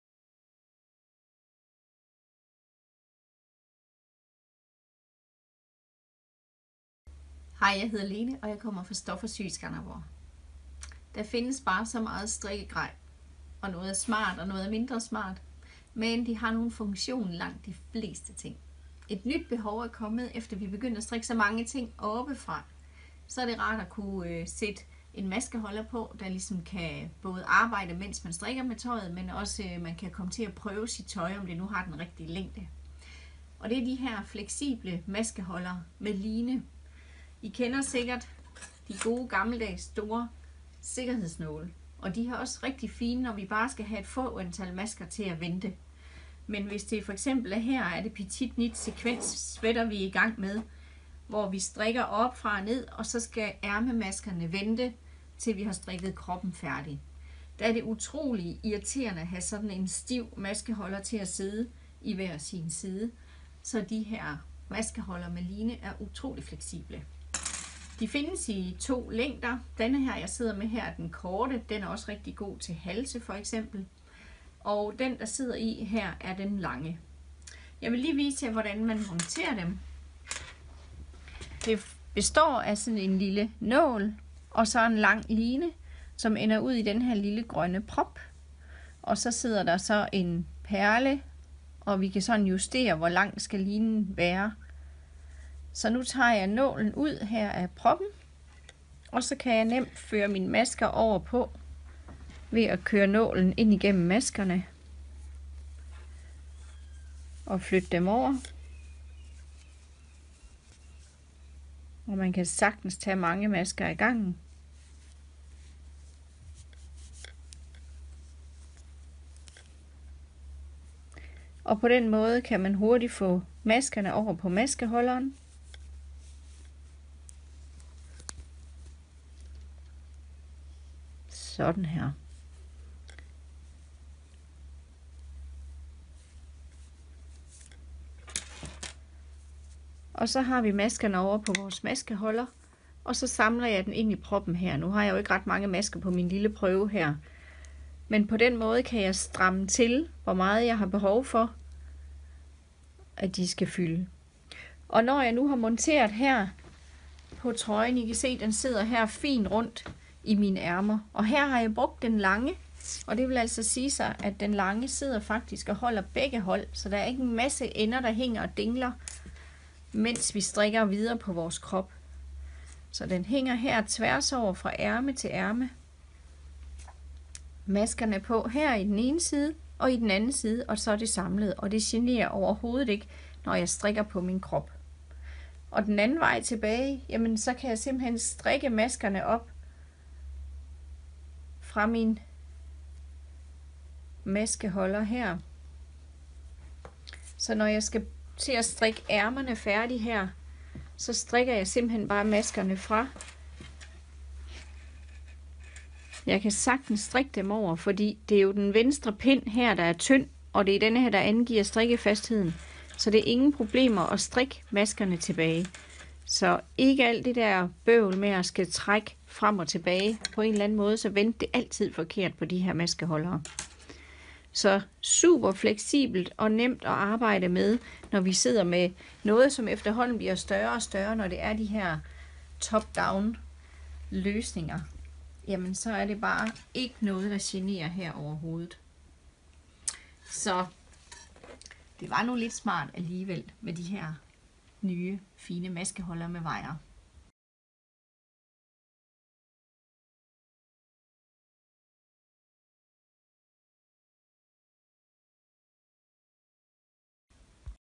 Clover maskeholder med line
Clovers maskeholder med line er genial til at pause masker, mens du strikker videre.
Lene viser her hvordan den virker.